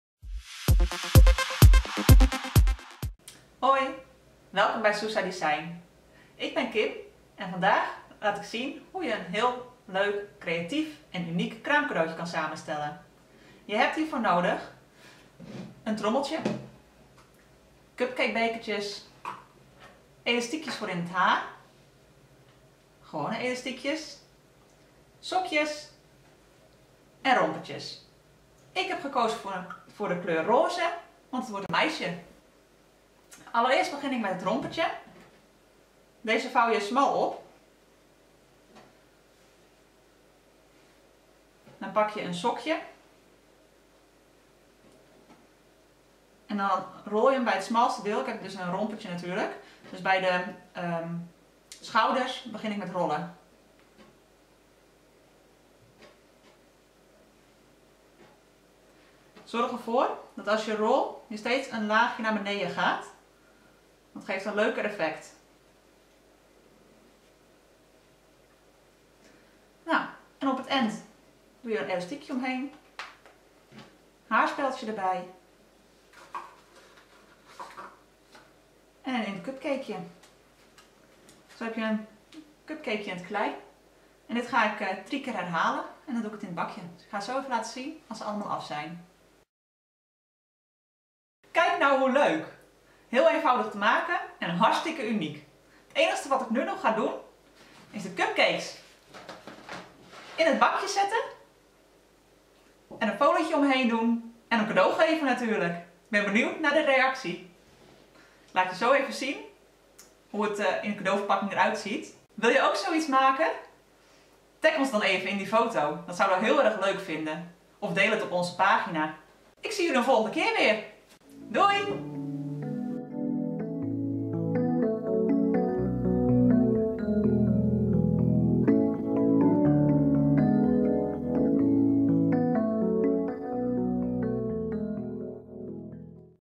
ZUSA-DESIGN | Origineel kraamcadeau
Geef je kraamcadeau op originele wijze door ze te verpakken als cupcakes! Bekijk de tutorial voor tips en tricks.

Je hebt hiervoor nodig:

- bakje
- babykleertjes (bijv. rompertjes)
- baby sokjes
- elastiekjes en/of speldjes
- papieren cupcake bakjes